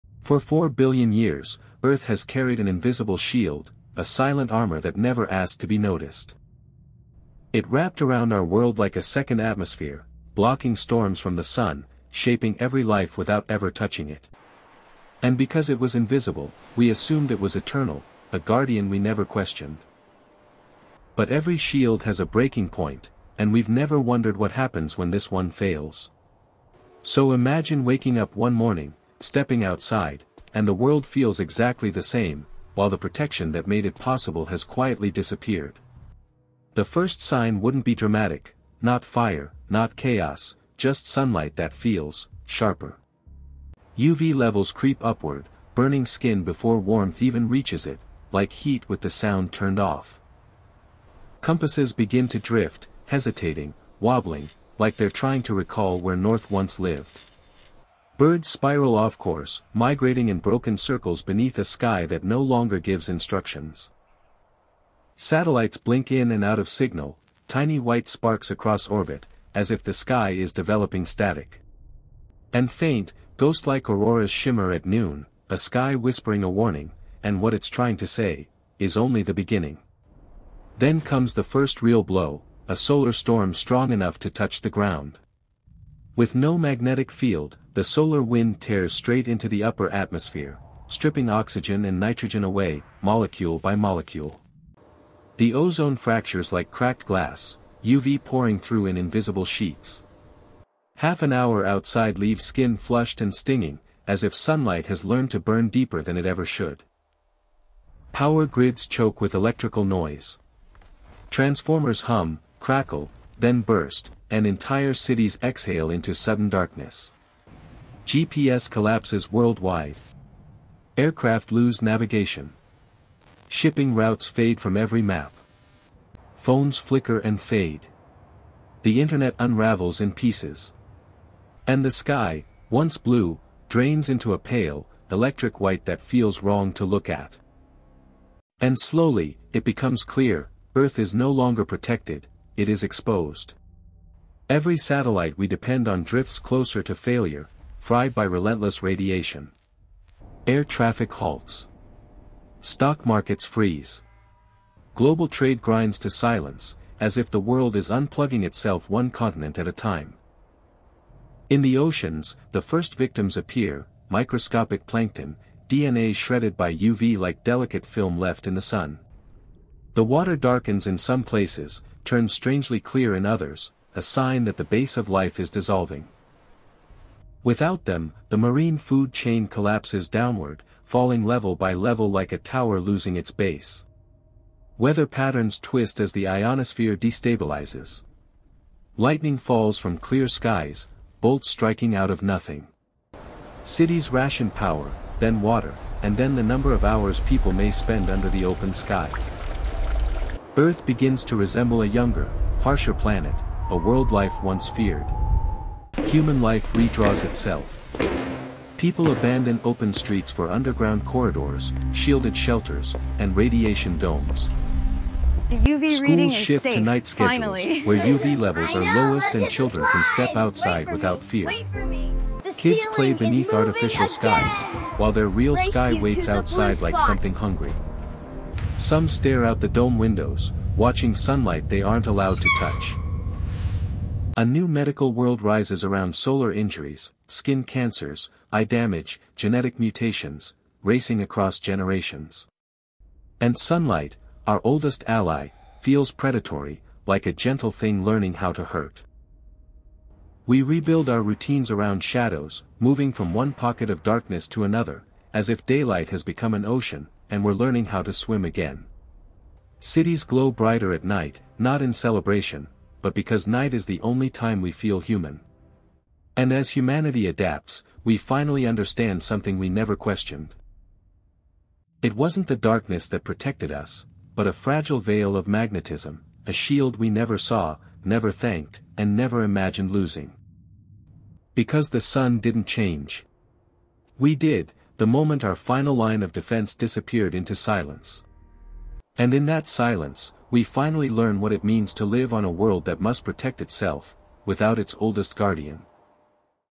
What If Earth’s Magnetic Field Suddenly Collapsed?
What if Earth’s magnetic field — the invisible shield that has protected life for billions of years — suddenly collapsed?

In this scientific speculative documentary, we explore what would really happen if our planet lost its only defense against solar storms, cosmic radiation, and the forces that shape life on Earth.
From failing satellites and collapsing navigation systems to dangerous UV exposure, ocean ecosystem breakdown, and the transformation of daily human life… this scenario reveals how fragile our world truly is.

This film is based on real scientific principles, atmospheric physics, and space-weather research. No exaggeration. No fiction. Only a realistic look at what the future could hold if Earth’s magnetic shield disappeared.

🌍 Nereus Core — Scientific Speculative Documentaries
Exploring the future through science, realism, and imagination.